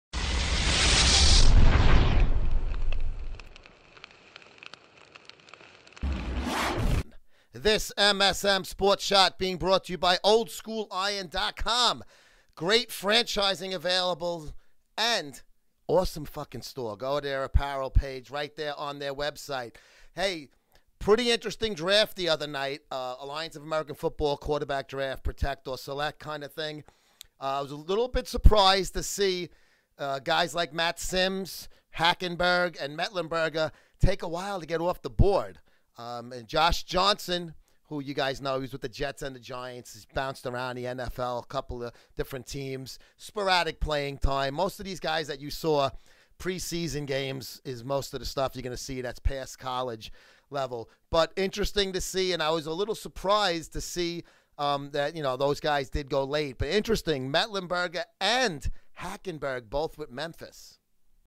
Alliance of American Football QB Draft Review - AAF
A quick review of the Alliance of American Football Quarterback Draft.

Image: AAF